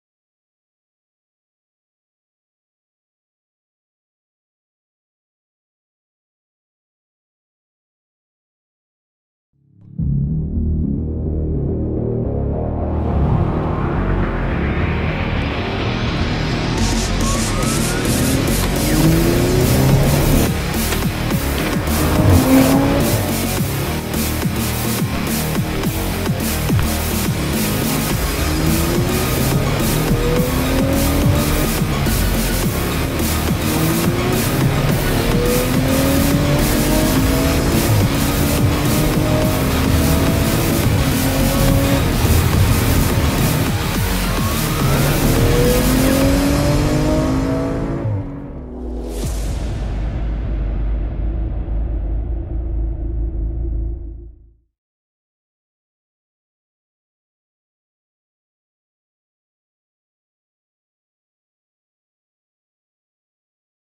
Chip tuning DTE Systems® BoostPro y Power Control
Descubre los chip tuning DTE systems Boostpro y Power control del gigante Alemán DTE Systems. Toda la información en:

Chip tuning: aumenta potencia y reduce consumos